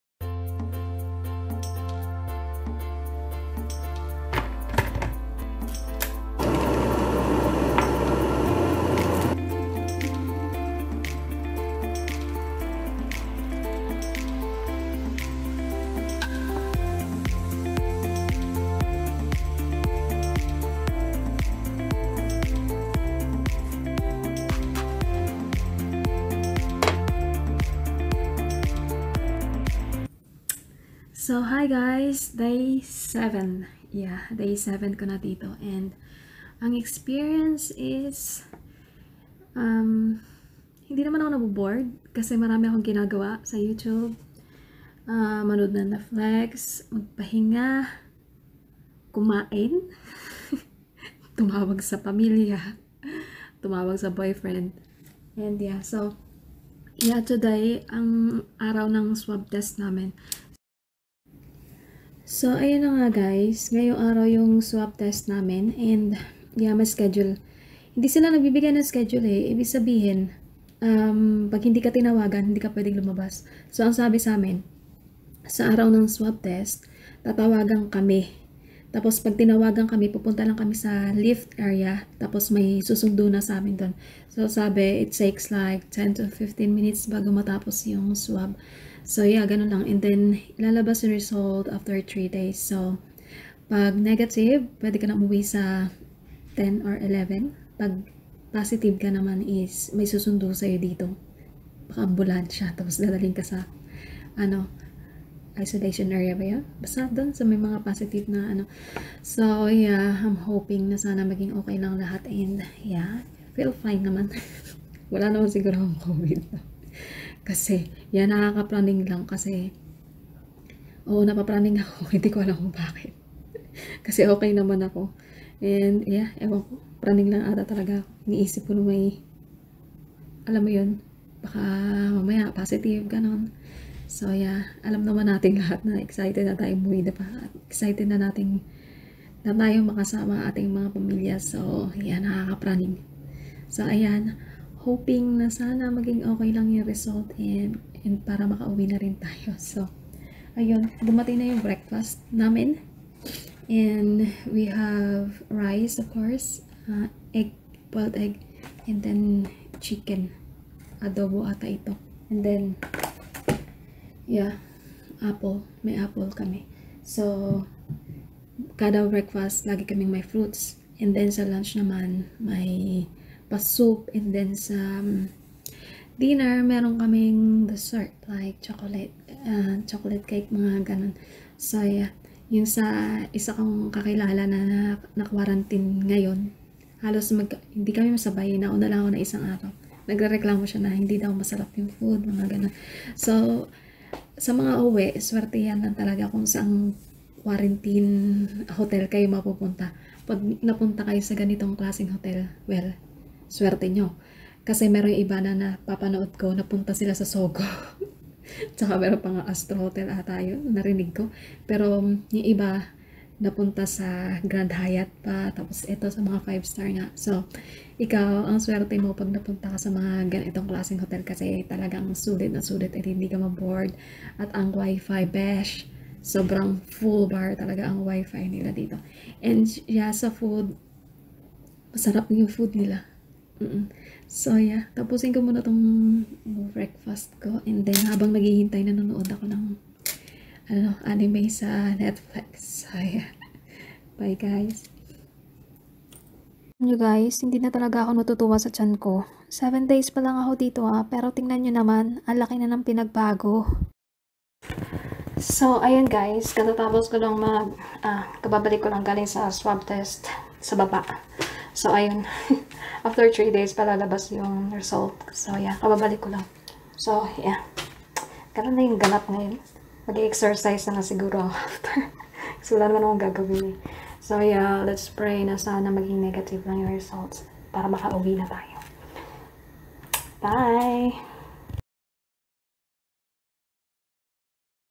DAY 7 AT SHANGRILA HOTEL|SWAB TEST AFTER 7 DAYS|OFW BALIKPINAS|cath haven
my day 7 at shangrila hotel..
swab test day na namin nung araw na to and mabilis lang sya.wala pang 10 minutes and hindi naman pala sya masakit tulad ng sabi nila..